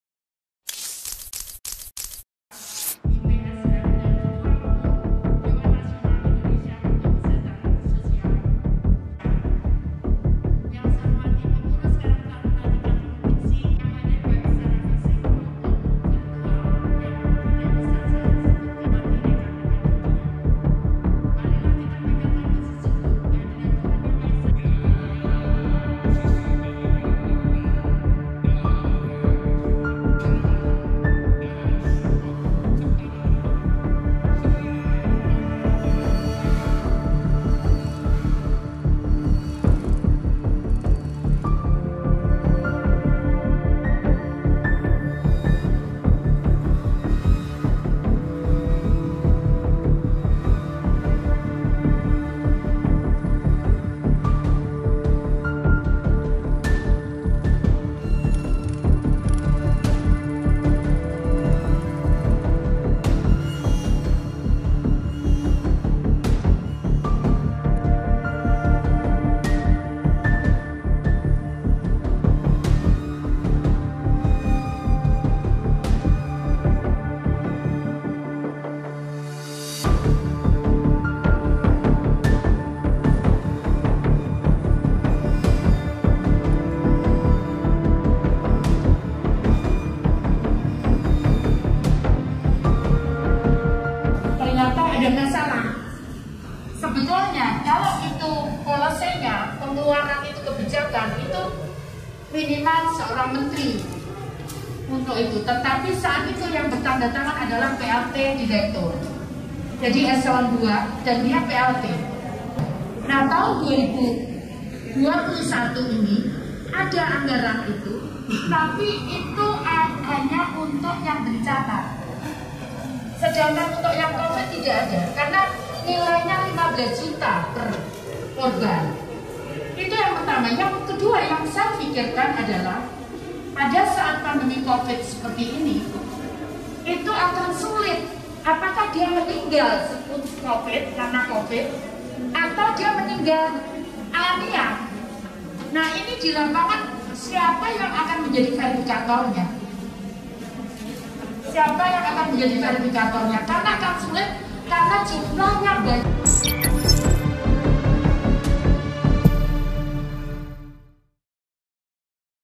Kemensos Hentikan Dana Santunan Kematian COVID-19
Kementerian Sosial menghentikan penyaluran santunan kepada korban meninggal akibat COVID-19 sebesar 15 juta rupiah per orang. Kebijakan itu dihentikan lantaran pada tahun 2021 anggaran Kementrian Sosial berfokus pada penanganan bencana.

Selain itu, Menteri Sosial Tri Risma Harini menilai ada dugaan pelanggaran yang terjadi dalam aturan pemberian dana santunan. Diantaranya aturan itu ditanda tangani oleh pejabat eselon dua yang berstatus pelaksana tugas sedangkan seharusnya ditanda tangani oleh pejabat setingkat menteri.

Risma menilai payung hukum tersebut sangat lemah untuk dijadikan pijakan dalam penerapan santunan sosial bagi para korban meninggal akibat COVID-19. Selain besarnya anggaran yang harus dikeluarkan dalam penerapannya sulit untuk mengidentifikasi korban yang meninggal akibat COVID-19 sehingga ditakutkan pemberian santunan tidak tepat sasaran. Metro TV/Rizki Nur Mohamad